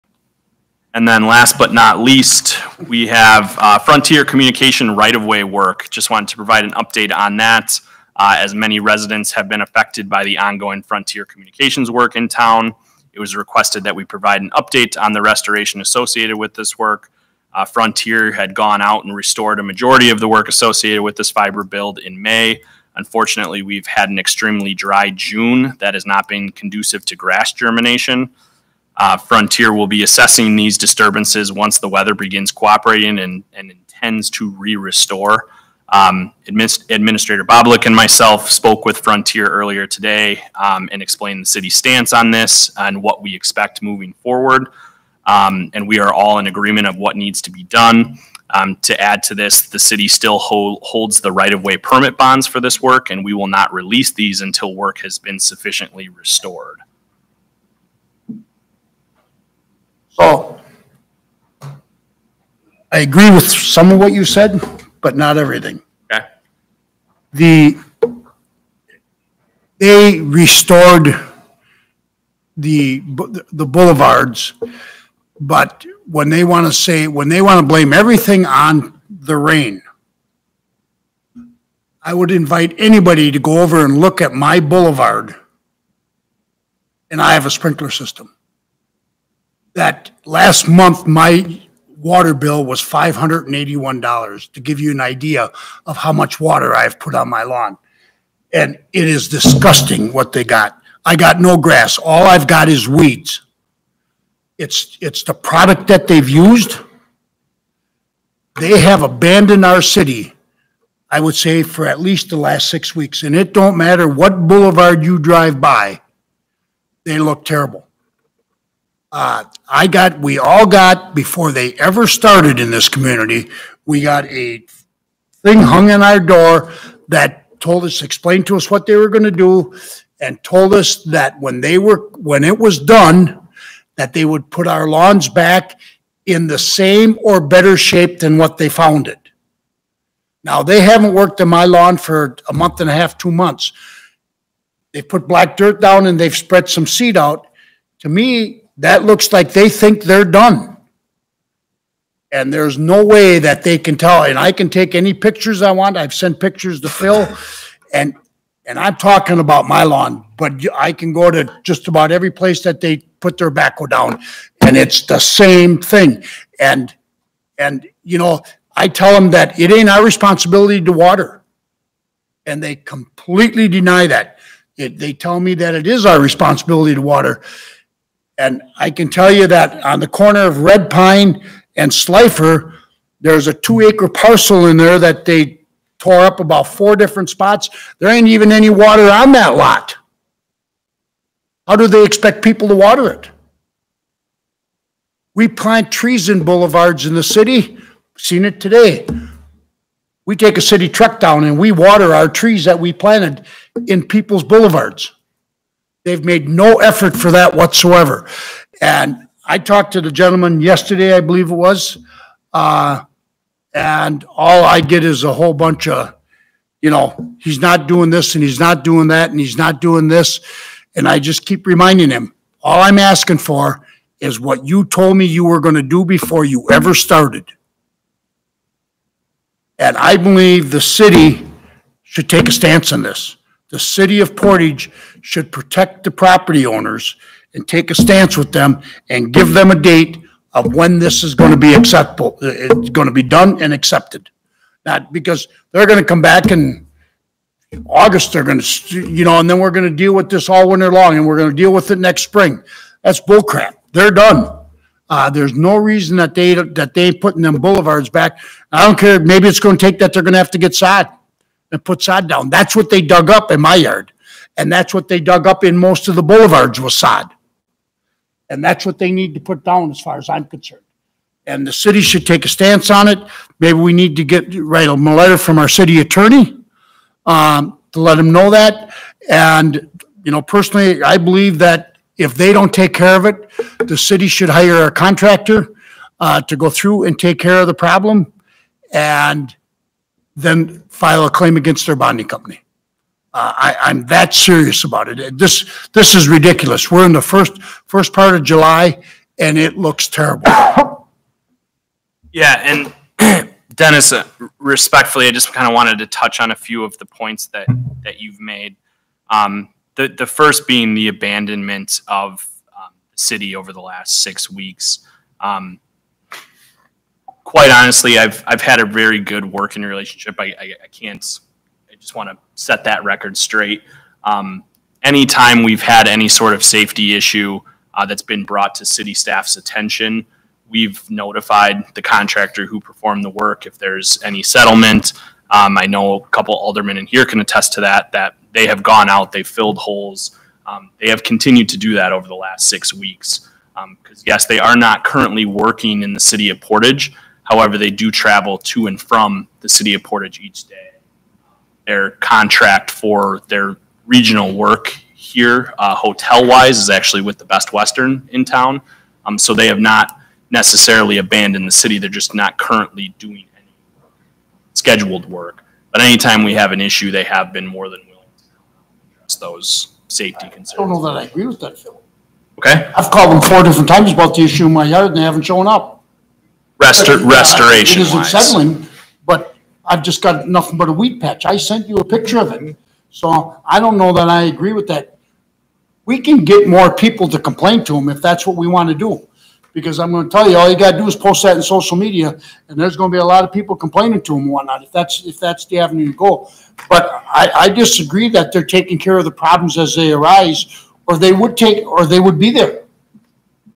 Portage - Municipalities meeting discussion on dead grass
Alder-person Dennis Nachreiner is unhappy with the dead grass after Frontier Communications did work in his boulevard.